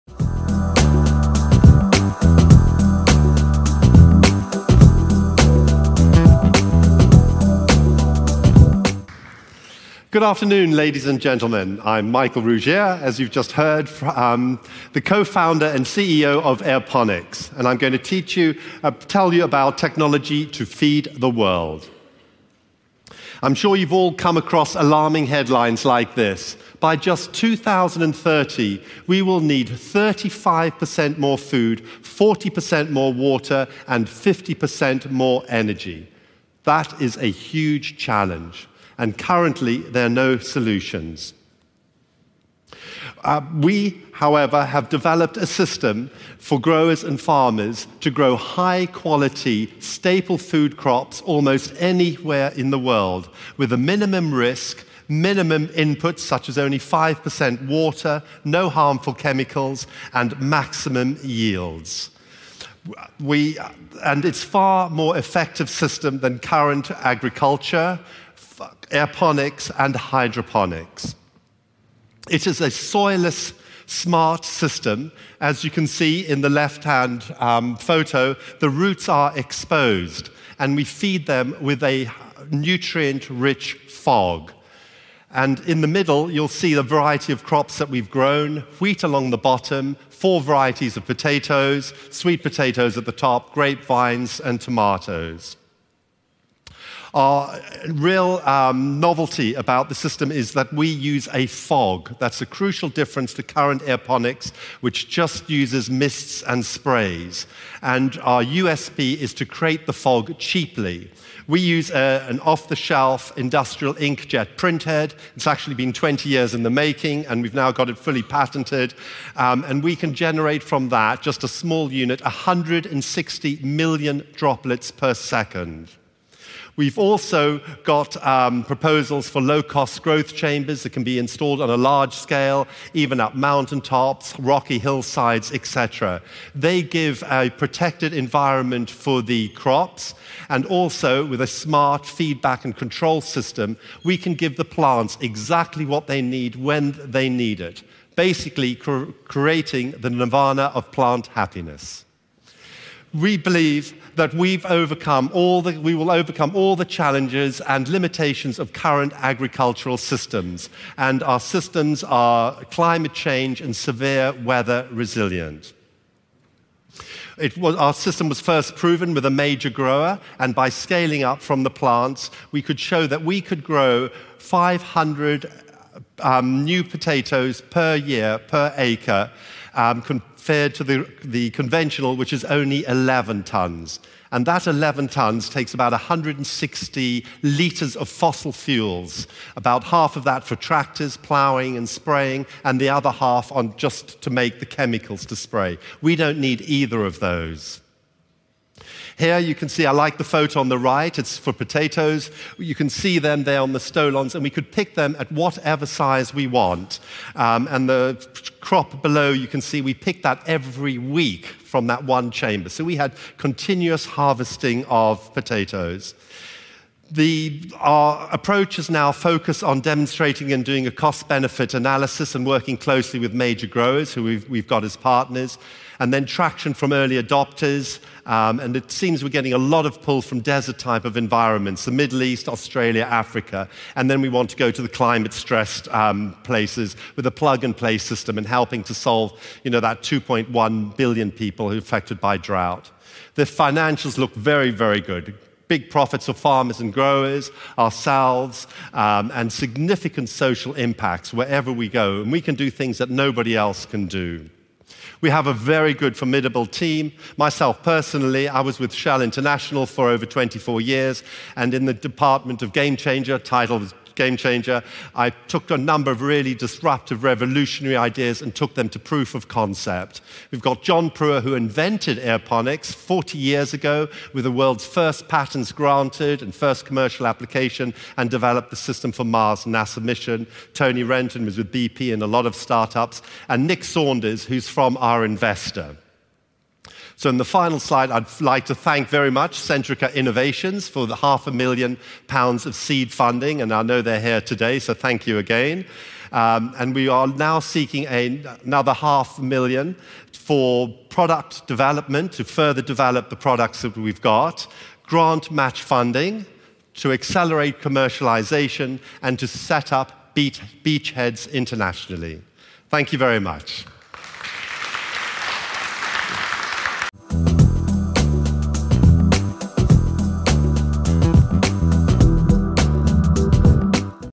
Michael Ruggier pitches Airponix
Nirvana of plant happiness: Michael Ruggier pitches Airponix at Ecosummit Berlin 2019. The UK startup provides advanced technology to feed the world. Their smart food growing system is soil-less and super water efficient. Airponix is backed by Centrica and currently fundraising.